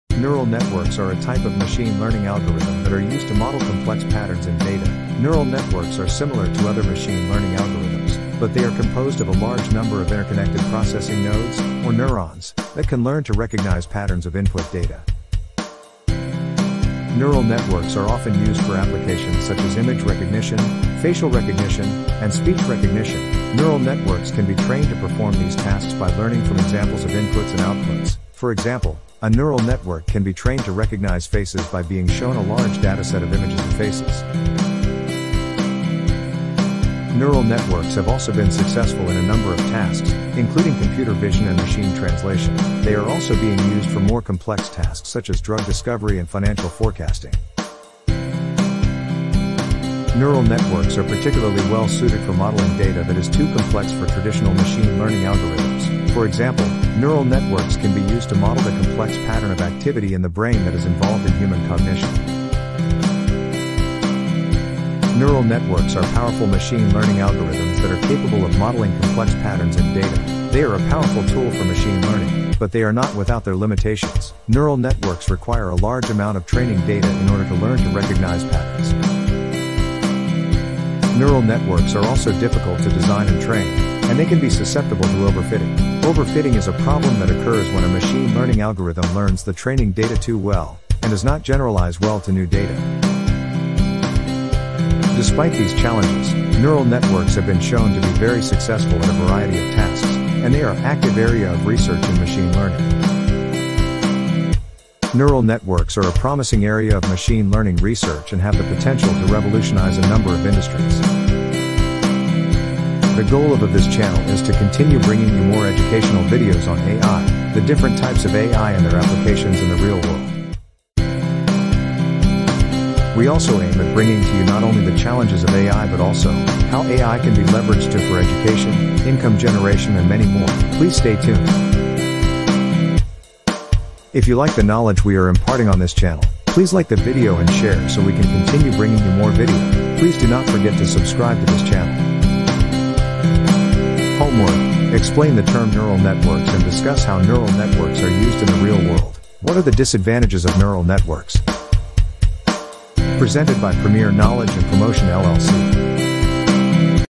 AI FOR BEGINNERS CHAPTER 10: NEURAL NETWORKS
Neural networks are a type of machine learning algorithm that are used to model complex patterns in data; Neural networks are similar to other machine learning algorithms, but they are composed of a large number of interconnected processing nodes, or neurons, that can learn to recognize patterns of input data.
Neural networks are often used for applications such as image recognition, facial recognition, and speech recognition; Neural networks can be trained to perform these tasks by learning from examples of inputs and outputs; For example, a neural network can be trained to recognize faces by being shown a large dataset of images of faces.
Neural networks have also been successful in a number of tasks, including computer vision and machine translation; They are also being used for more complex tasks such as drug discovery and financial forecasting.
Neural networks are particularly well suited for modeling data that is too complex for traditional machine learning algorithms; For example, neural networks can be used to model the complex pattern of activity in the brain that is involved in human cognition.
Neural networks are powerful machine learning algorithms that are capable of modeling complex patterns in data; They are a powerful tool for machine learning, but they are not without their limitations; Neural networks require a large amount of training data in order to learn to recognize patterns
Neural Networks are also difficult to design and train, and they can be susceptible to overfitting; Overfitting is a problem that occurs when a machine learning algorithm learns the training data too well, and does not generalize well to new data.
Despite these challenges, neural networks have been shown to be very successful at a variety of tasks, and they are a active area of research in machine learning.
Neural networks are a promising area of machine learning research and have the potential to revolutionize a number of industries.
Free AI Course for Beginners, Free AI Course